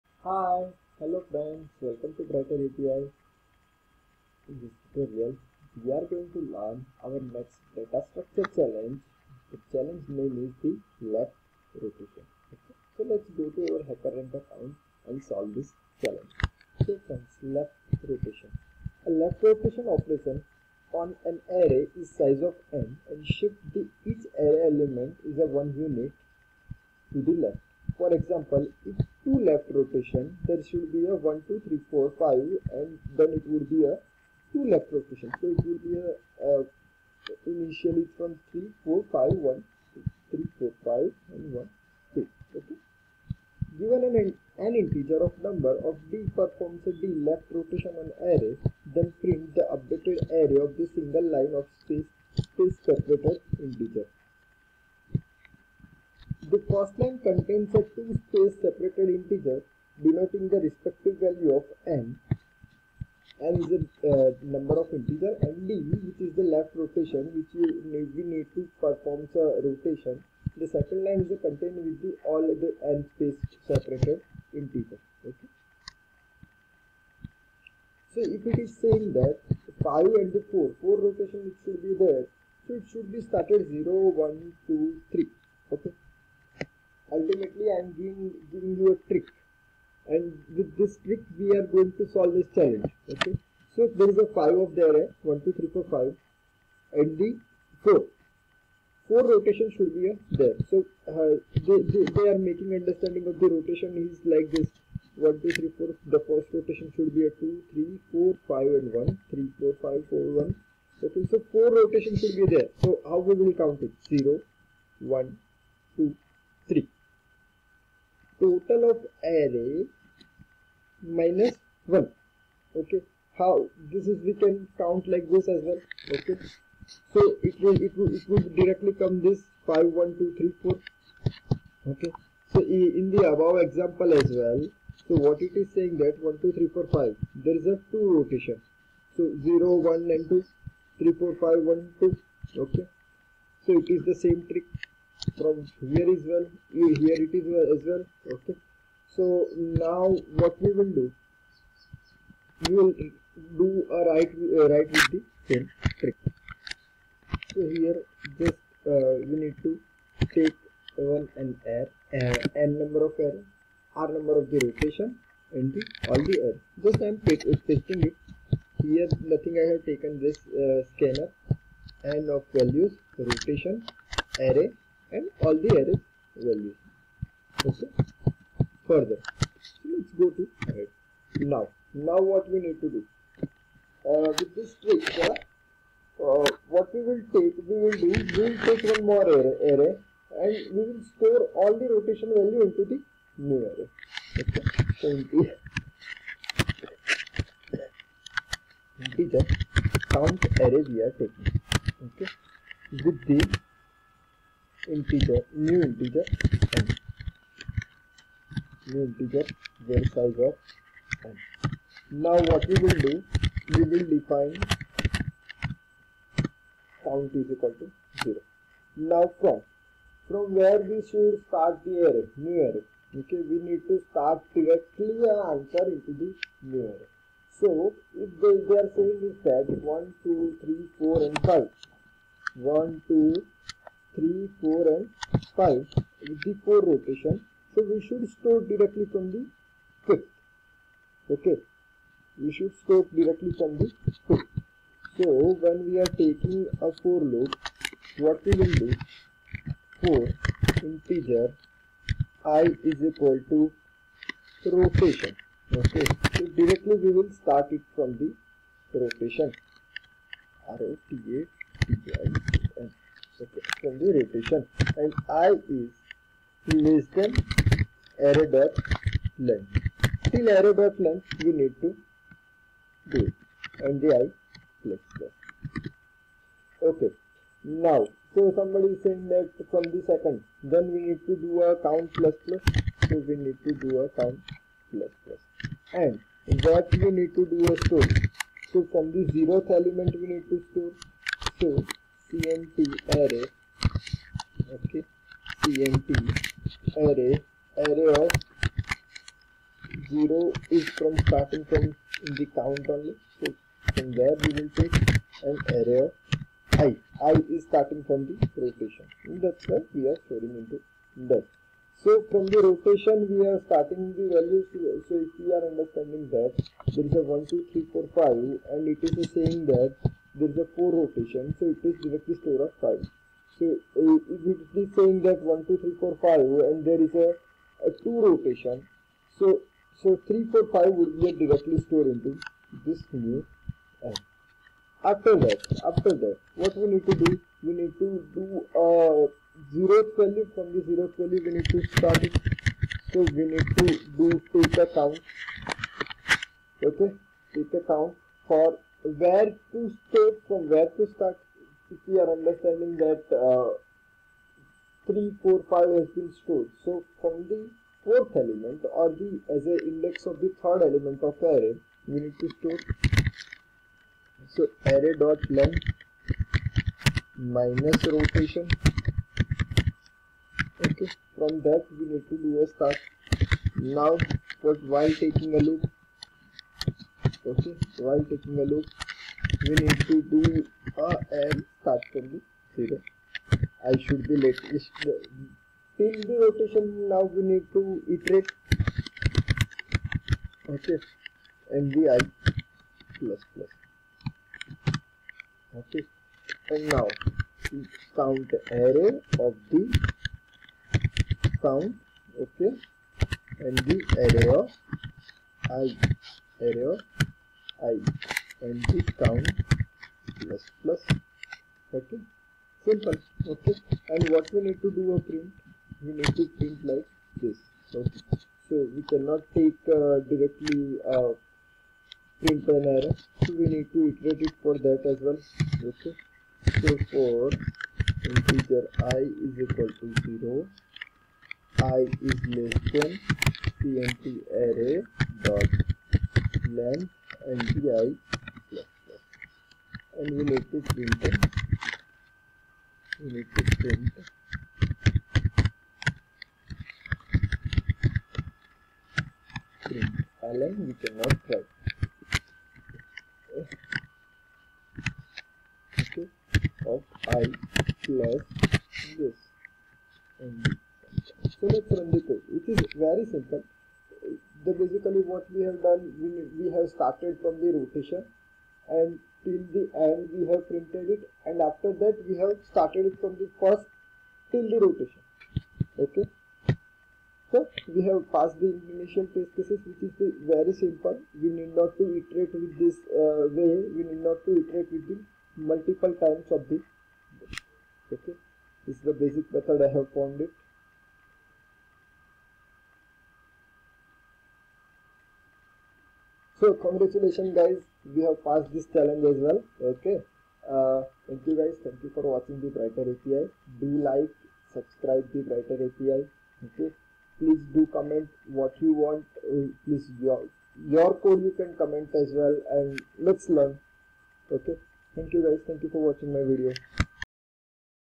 Hackerrank Data Structure Arrays - Left rotation Solution in Java
Hello Friends In This Tutorial We are going to crack our next Hackerrank data stucture Problem  of Arrays section is Left rotation.